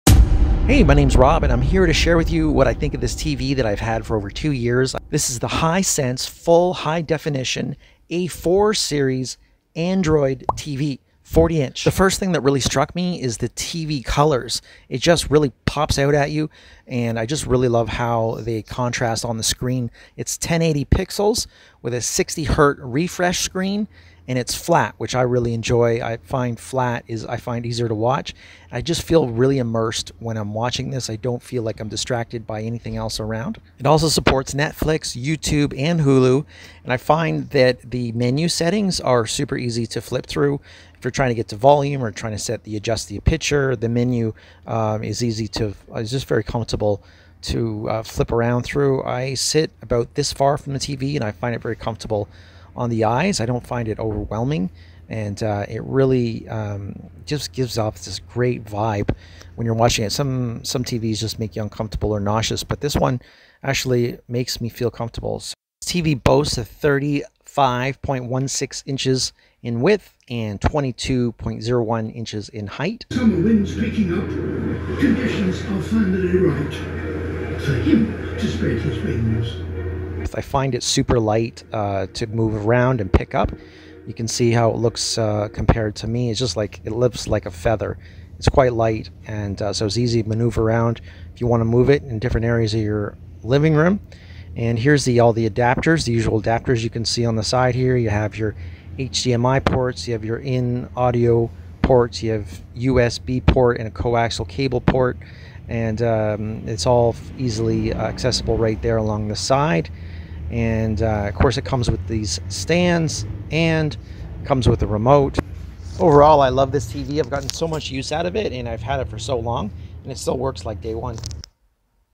I used the Hisense full hd A4 Android Tv Flat 40inch flat screen for 2 years. my thoughts...review
I used the Hisense full hd A4 Android Tv Flat 40inch flat screen for 2 years. my thoughts...